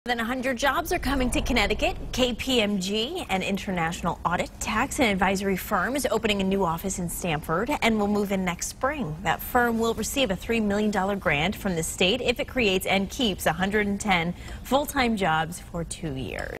KPMG to open new office, bringing 110 jobs
An international audit, tax and advisory firm is opening a new office in Connecticut and bringing 110 jobs.